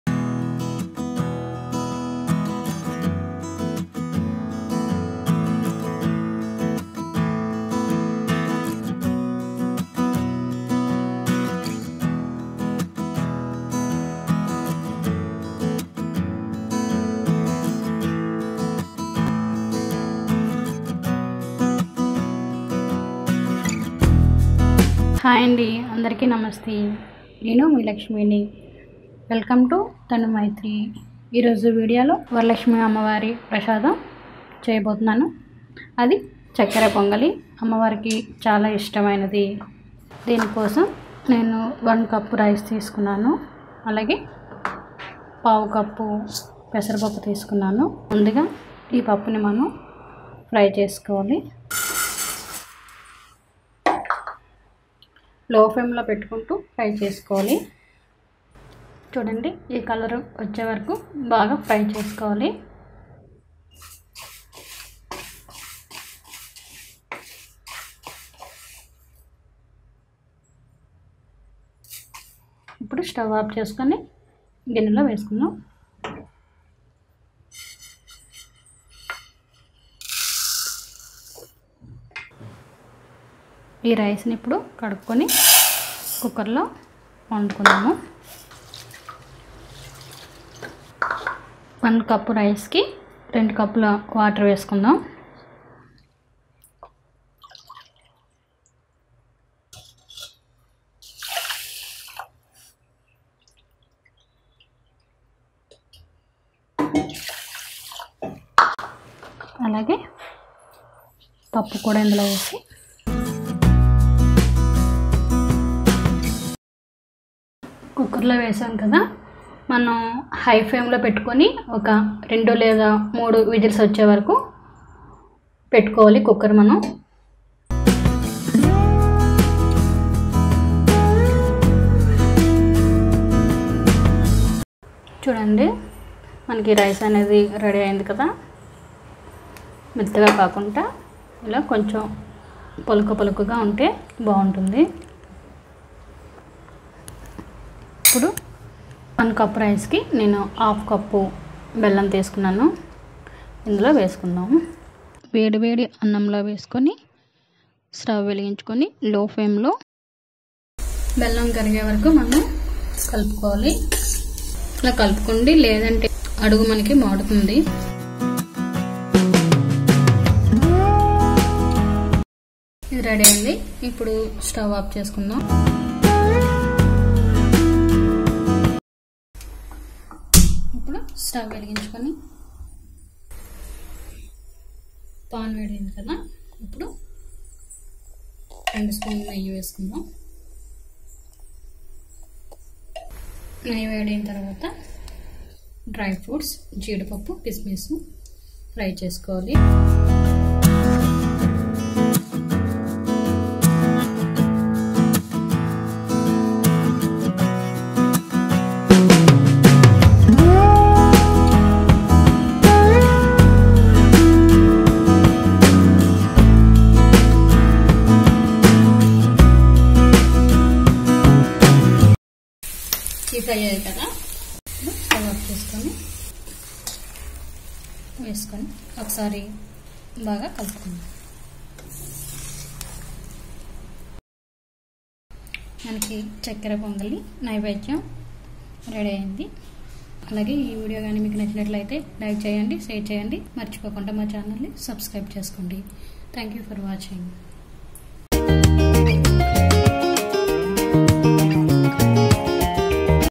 అమ్మవారి ప్రసాదం చక్కెర పొంగలి | Chakkara Pongali Recipe in Telugu | Tanumytreyi
అమ్మవారి ప్రసాదం చక్కెర పొంగలి తెలుగులో | Chakkara Pongali Recipe in Telugu | Tanumytreyi
                                   *** Welcome to Tanumytreyi ***